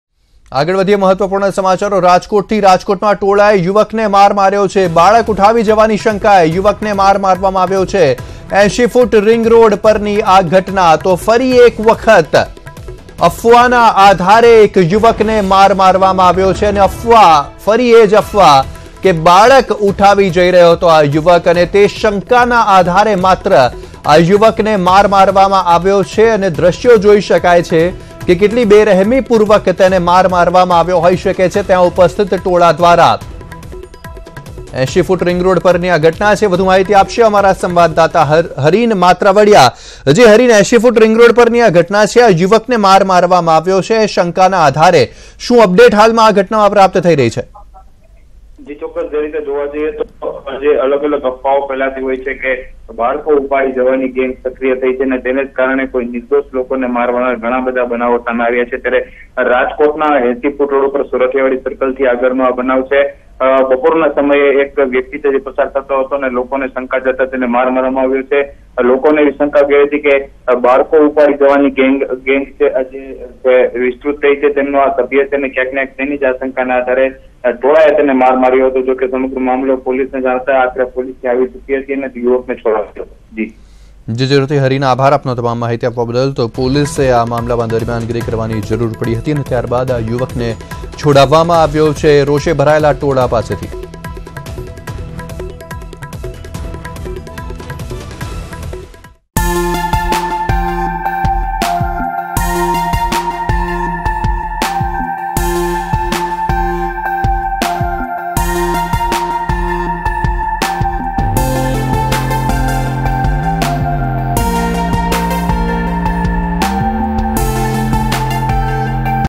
Rajkot માં ટોળાએ યુવકને માર્યો ઢોર માર | News18 Gujarati
Rajkot માં ટોળાએ યુવકને માર્યો ઢોર માર. Rajkot: Mob thrashed youth in doubt of child theft.